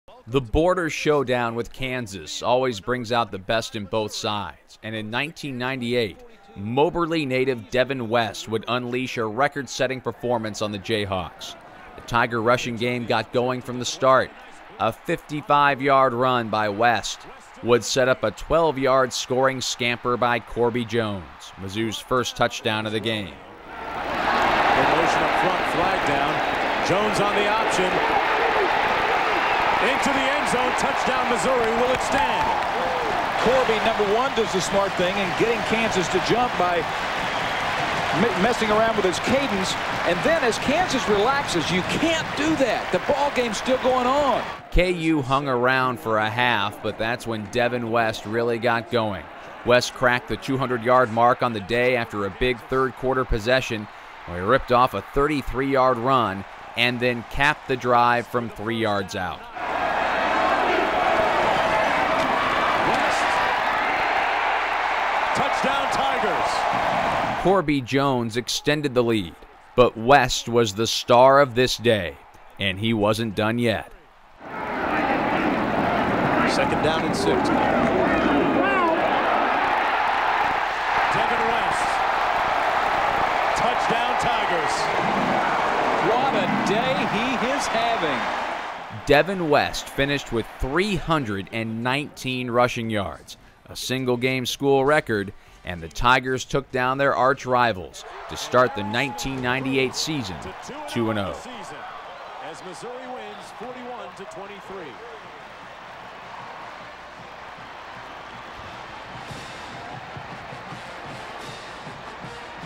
FLASHBACK: Devin West's record-breaking day vs Kansas in 1998
Devin West ran all over the Jayhawks in 1998 as the Mizzou senior broke the school's single-game rushing record with 319 yards against Kansas.  This is a production of the Mizzou Network.  For more videos follow Mizzou Football and Mizzou Network on Facebook, Twitter and Instagram.